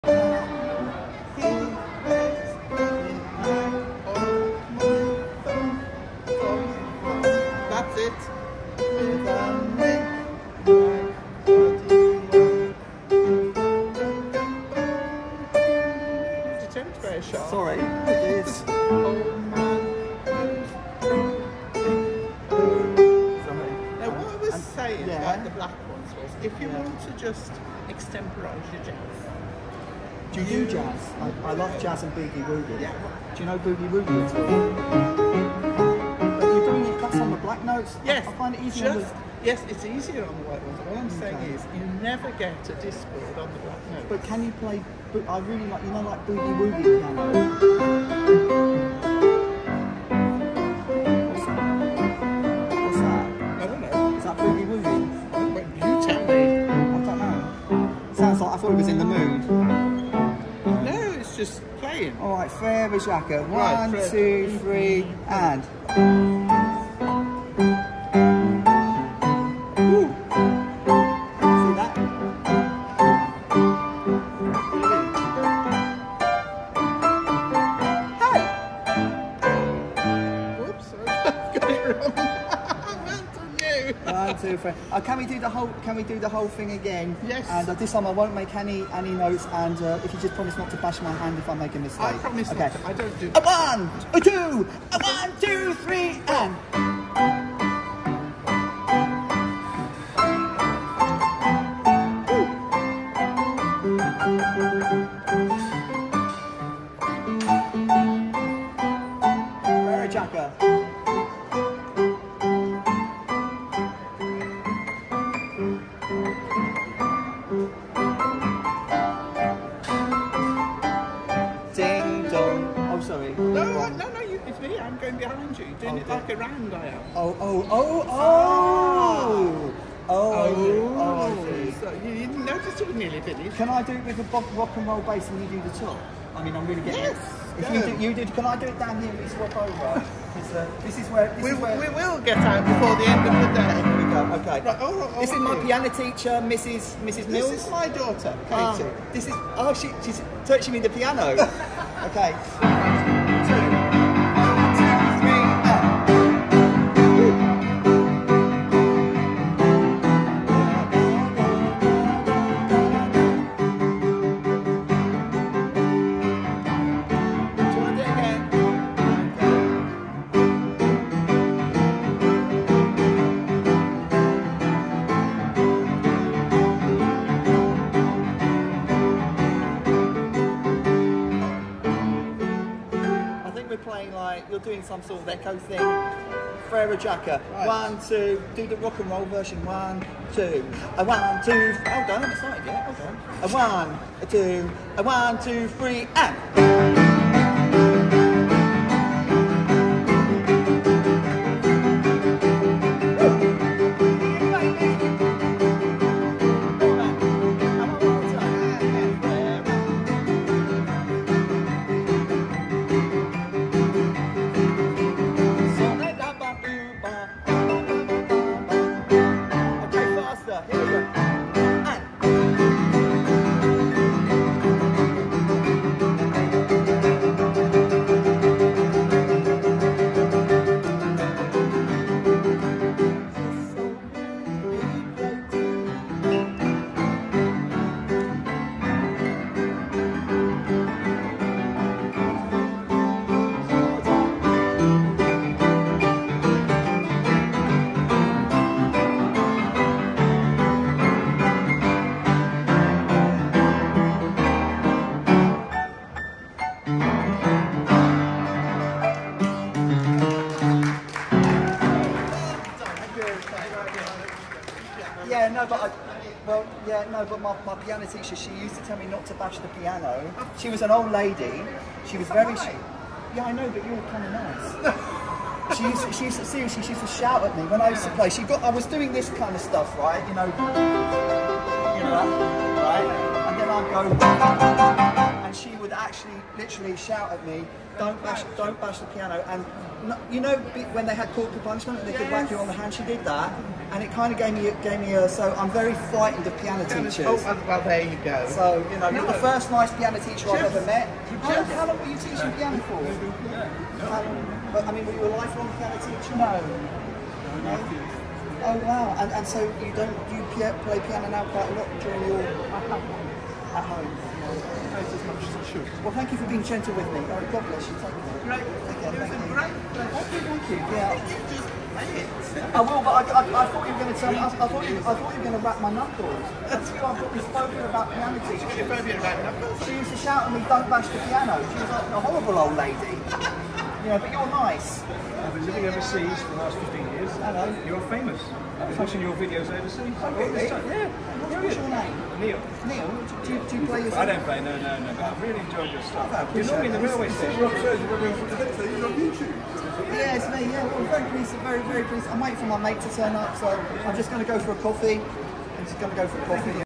Music Teacher Tutors Boogie Pianist On The Basics
Old school piano teacher shows Dr K the basics.